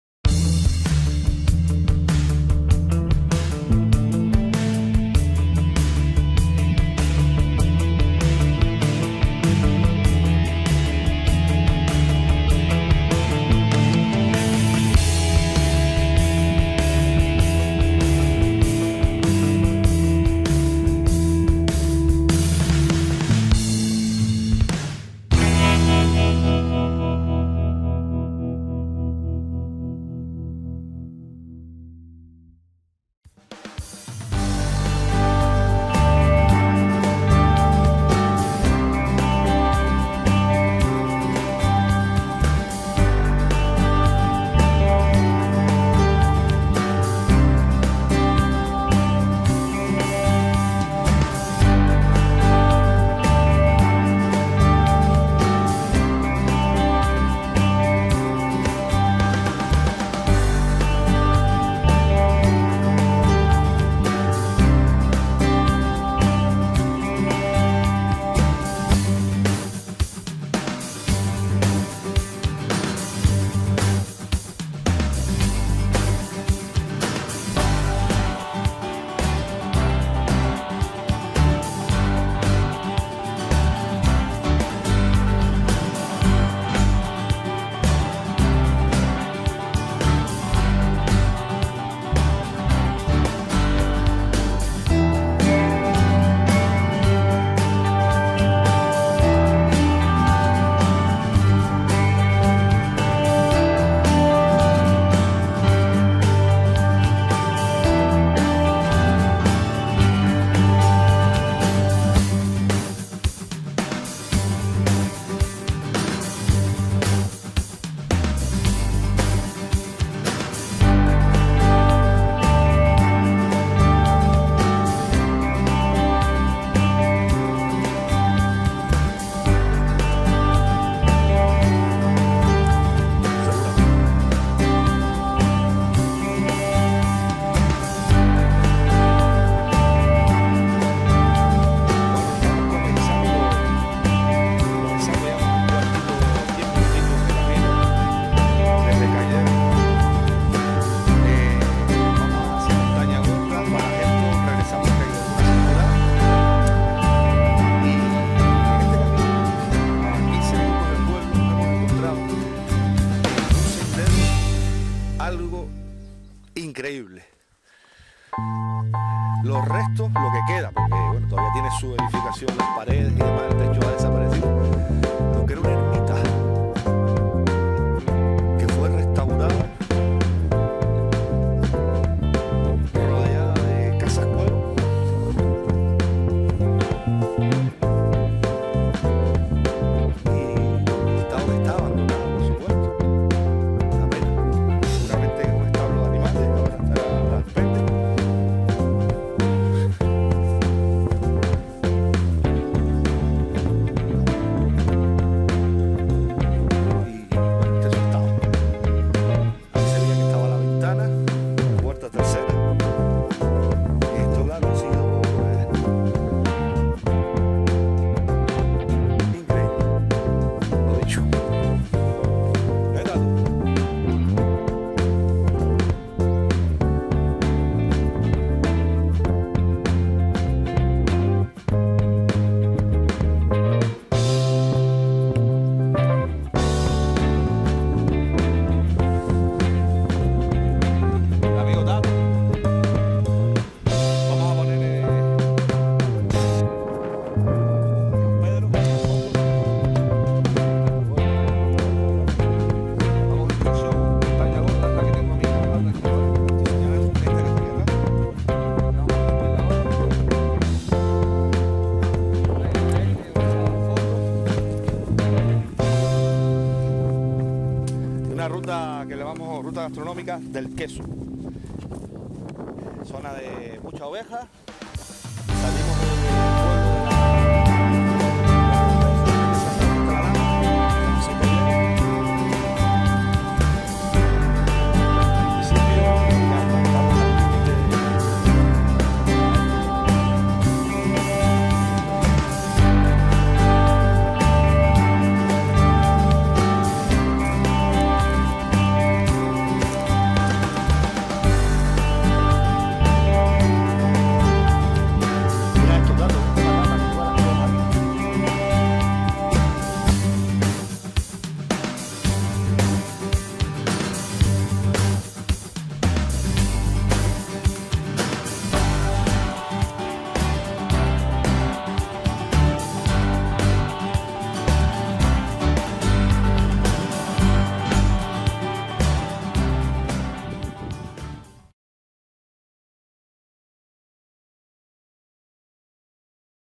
ruta de pateo circular caideros de gáldar montaña gorda fagajesto
La ruta comienza en el pueblo de San José de Caideros el cual a la salida dirección a la cumbre tomamos un camino serventia vecinal el cual nos lleva a la ermita chica. De ahi pasando por las casas cuevas el camino transcurre en dirección al valle de Agaete pero sin bajar mucho, en un desvío a la izquierda en el zic zac del sendero cogemos en dirección cumbre visualizando los roques de San Pedro y la cabecera del valle. este seguira voldeando hasta llegar a la altura de cortijo de caidero y siempre visualizando la montaña o volcán de fagajesto que es nuestro objetivo.

Grupo de Senderismo Archipiélago Canario.